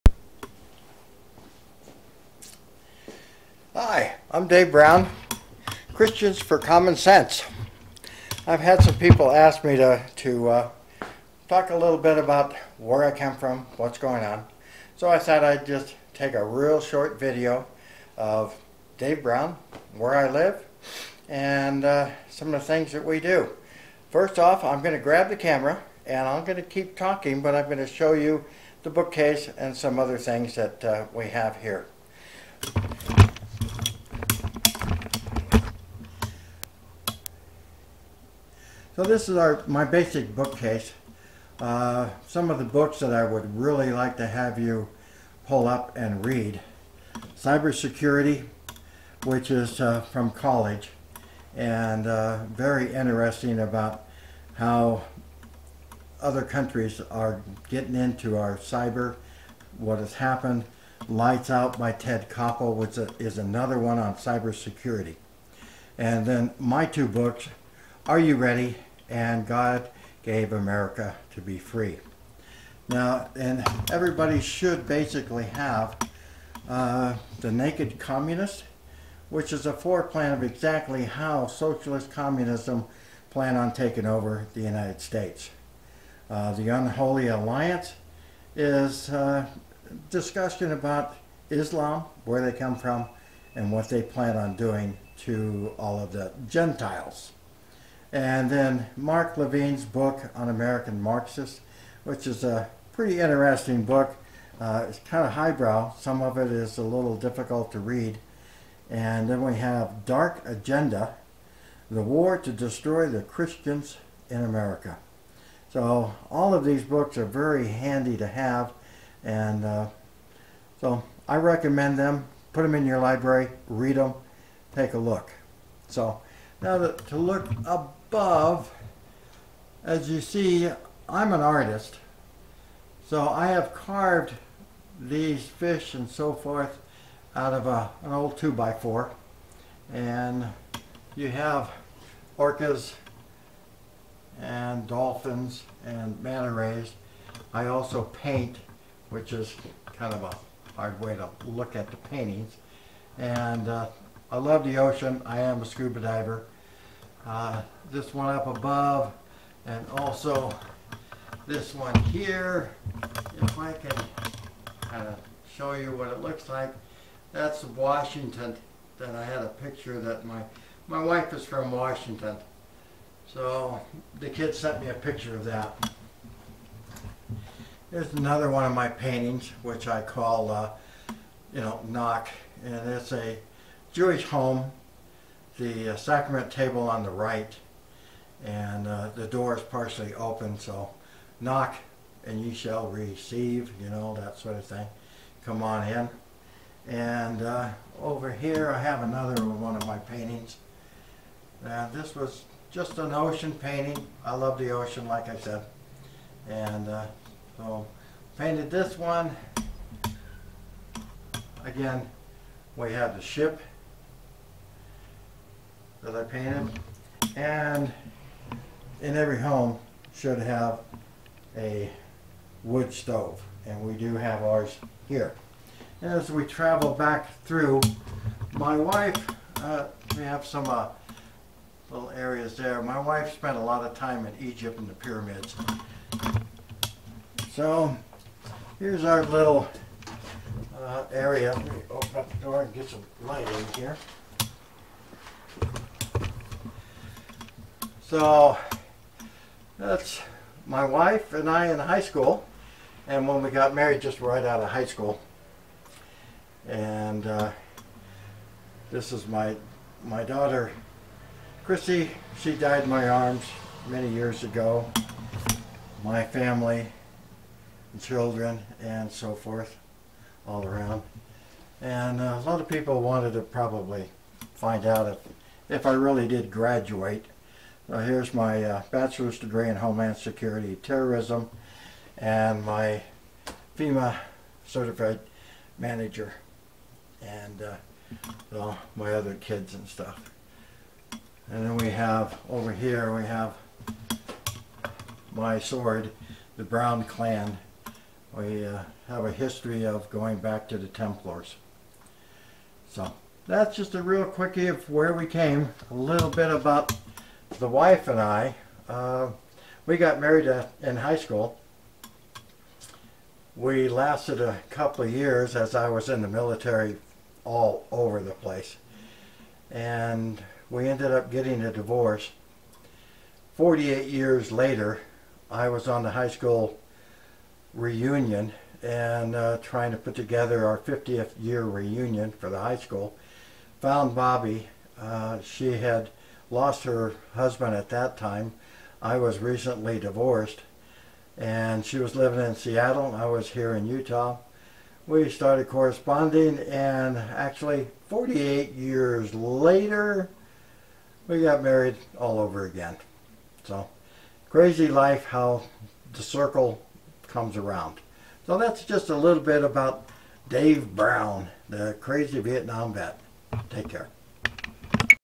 My Home. Just a look around who Dave Brown is.
Who is Dave Brown? Just a look around my small home. The kids have all gone and the wife and I have downsized the home. A look at my art and just who I am.